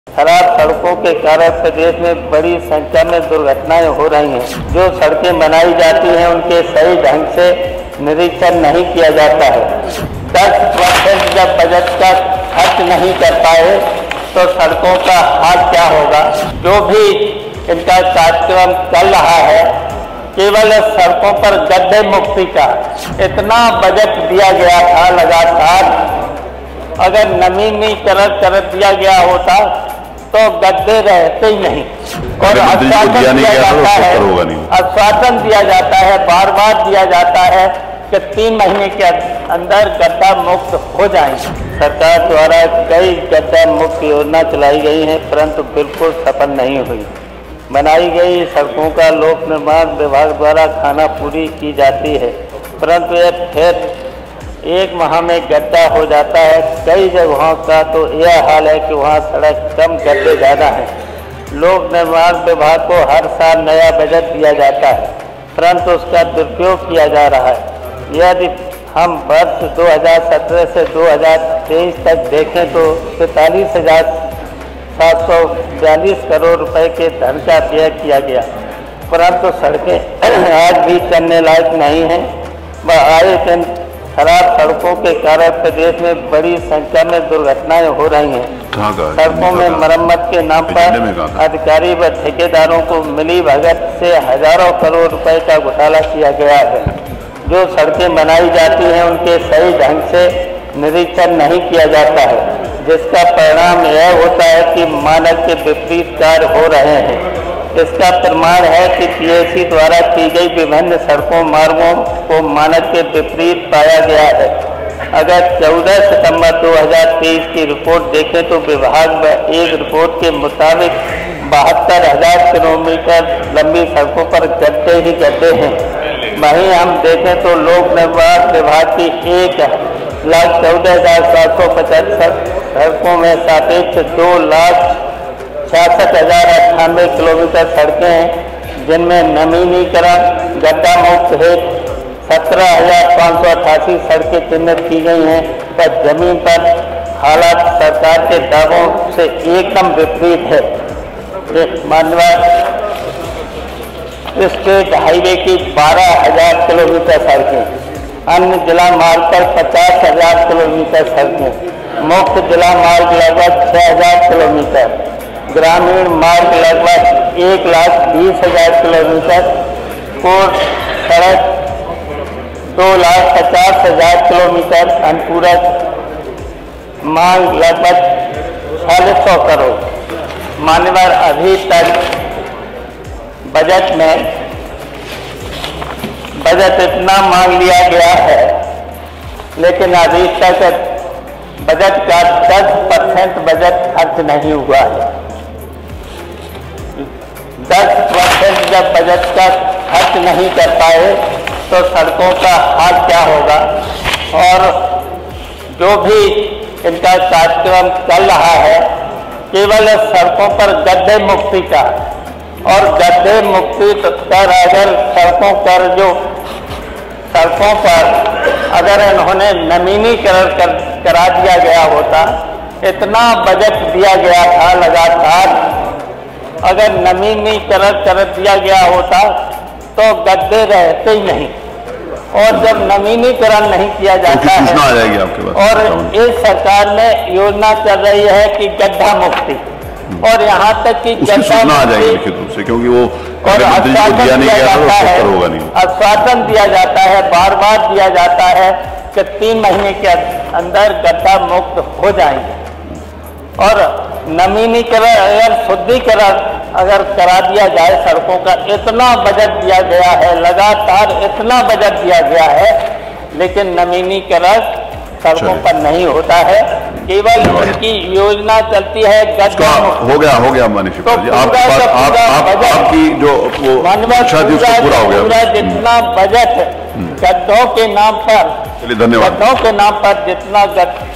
विधानसभा में Shivpal Yadav ने योगी सरकार को धो डाला | UP Vidhan Sabha Live | The Rajneeti
विधानसभा में शिवपाल यादव ने योगी सरकार को धो डाला, बाते सुन कर योगी हो गया परेसान.